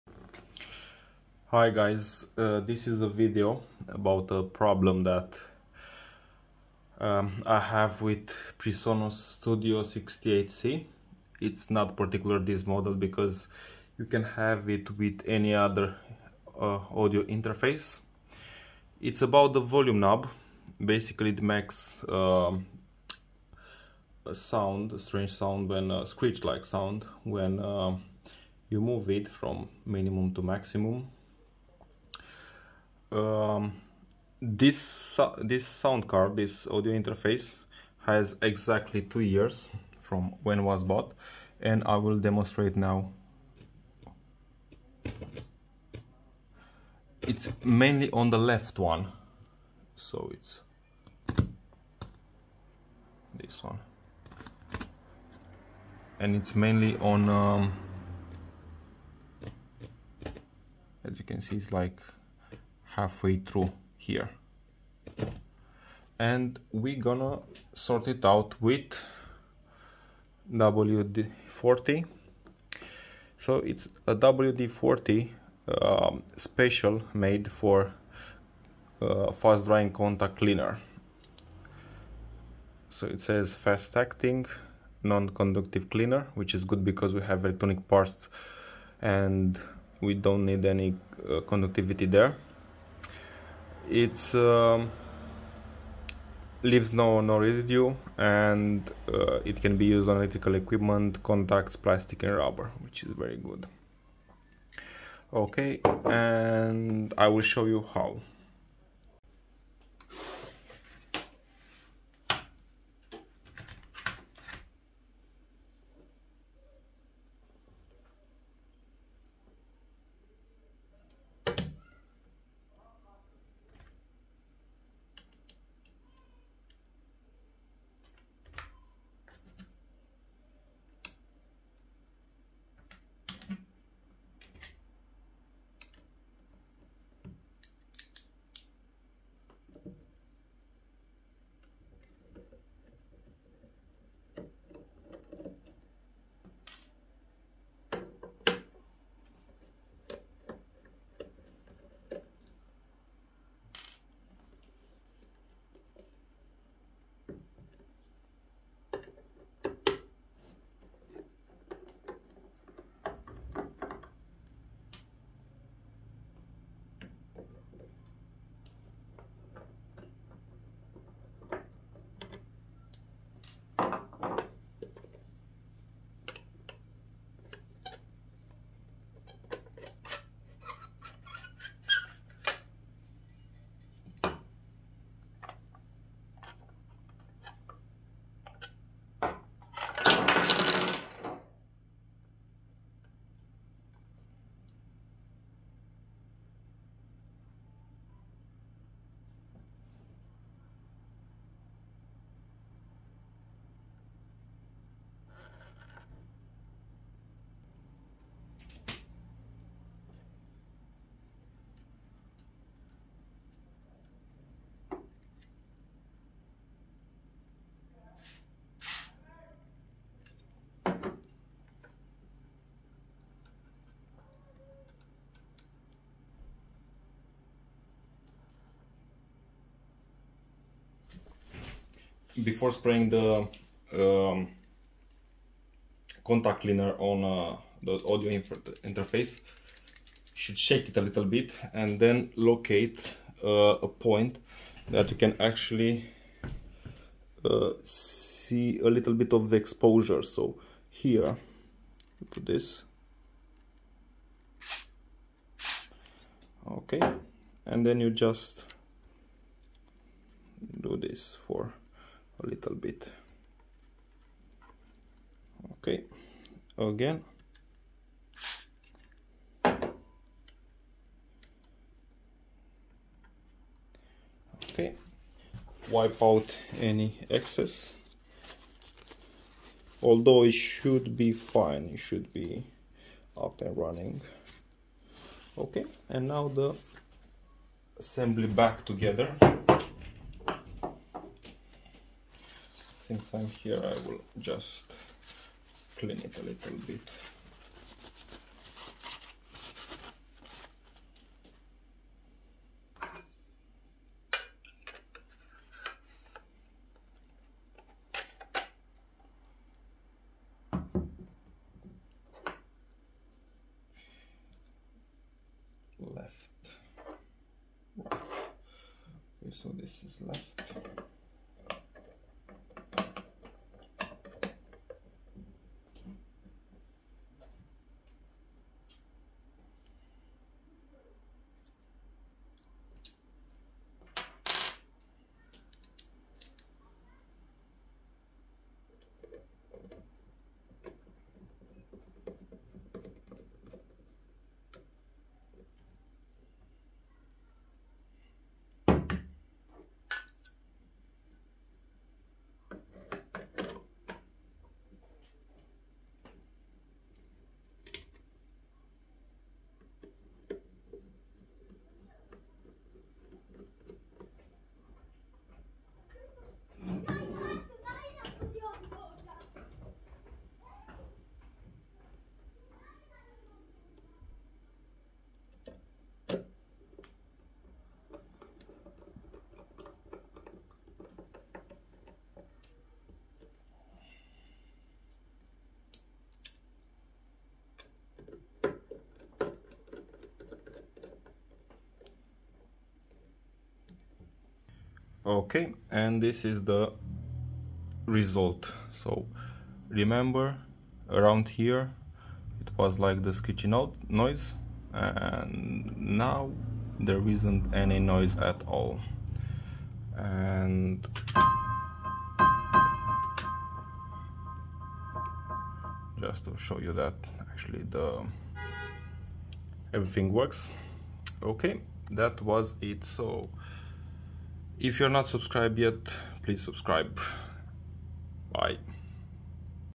How to get rid of screeching sound that volume potentiometers make: Cheap and effective solution!
In this video, you can see how to disassemble Presonus Studio 68c and clean the volume potentiometer which makes a screeching sound when actioned. This is due to the dust that enters the metal body of the potentiometer and prevents it to slide as it should on the semiconductor surface.  A screeching or crackling noise is something you really want to get rid of so we have the easy fix.
This fix can be applied at any Audio Interface or Power Amplifier etc. For this, we used a WD-40 Specialist Contact Cleaner which works perfectly but there are other alternatives if you cant find this particular one.

Video contents:
00:00 Intro
00:41 Demonstrating the problem
01:12 WD-40 Contract Cleaner
01:49 Dissansebly of Audio Interface
03:51 How to spray the contact cleaner
05:03 Assembly of Audio Interface
06:35 The result

If you would like to follow our activity,